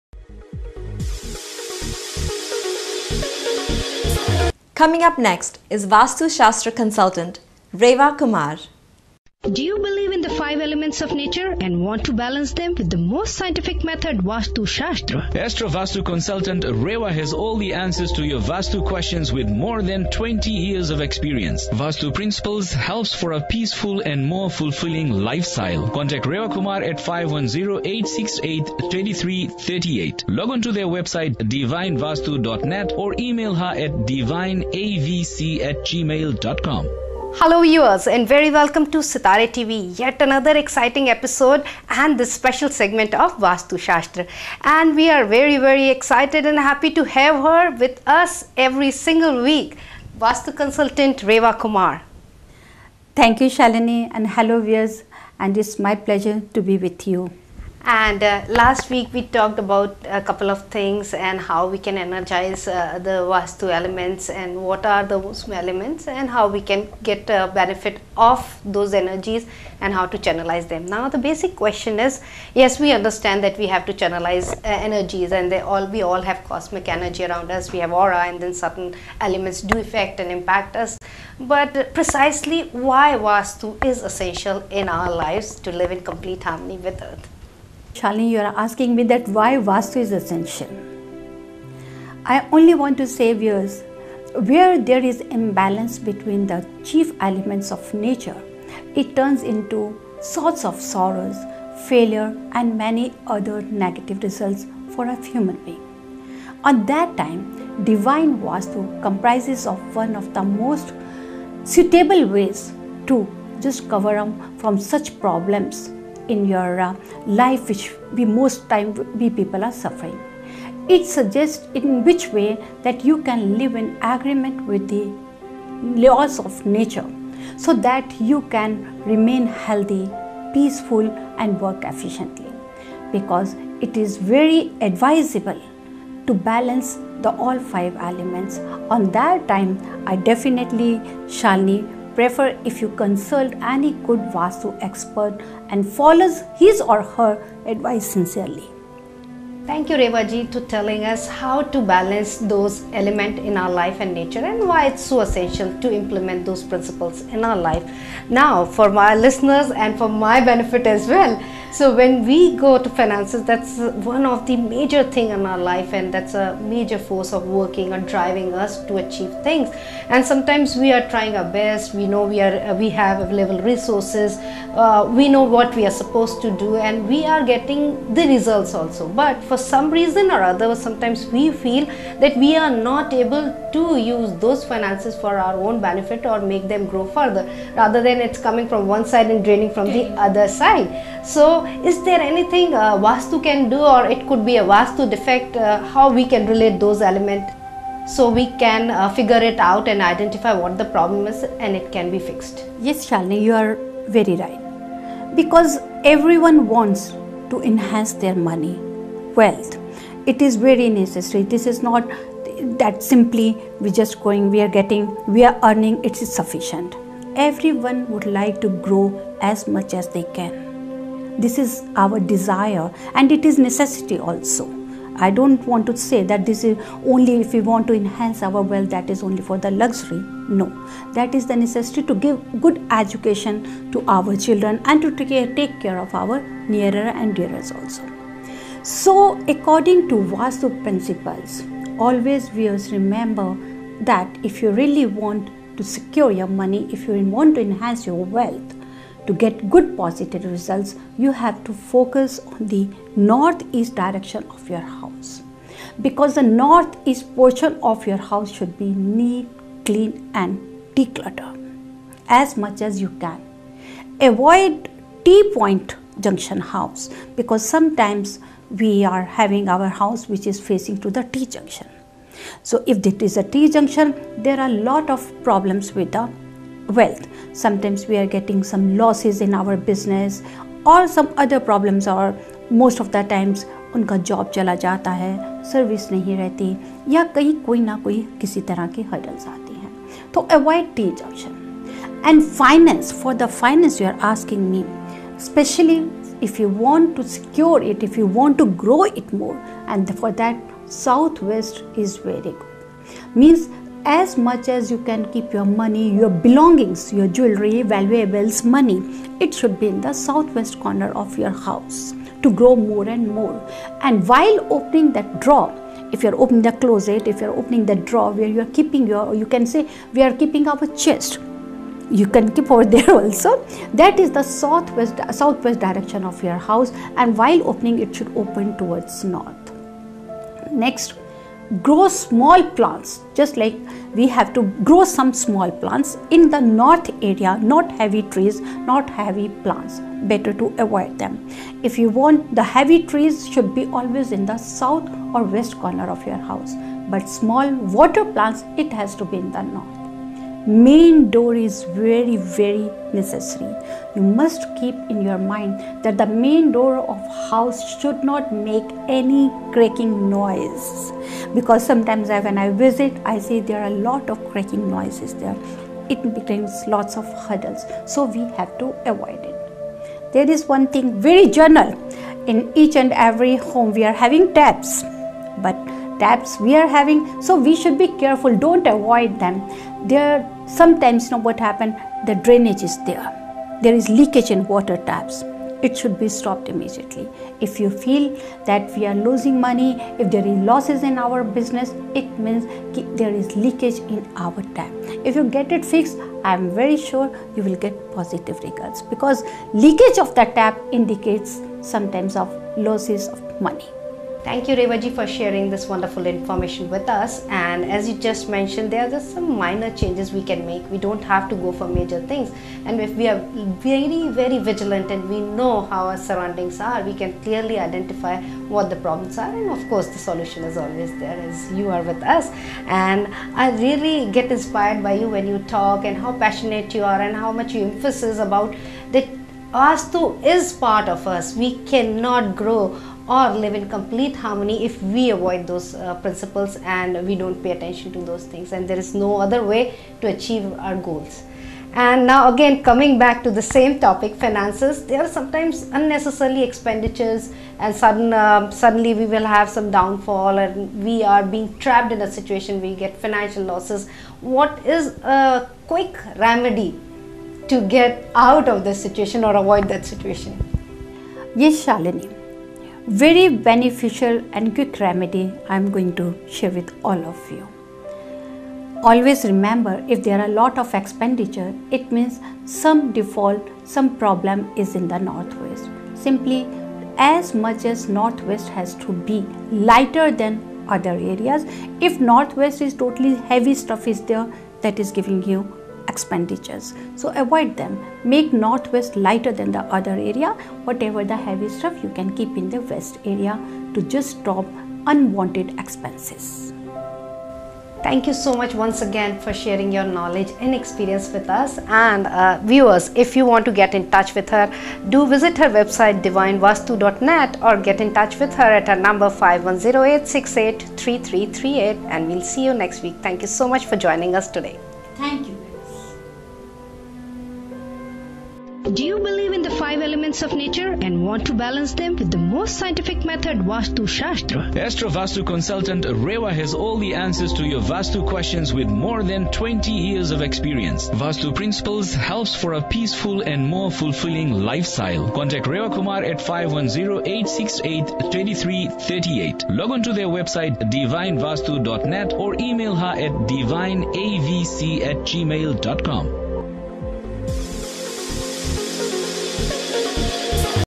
Rewa Kumar Divine Vastu Consultant Talk Show on Sitaarre TV (KTSF26) USA on Feb 17, 2013
Talk Show on Vastu Shastra; Sitaarre TV (KTSF26/ USA) Feb 17, 2013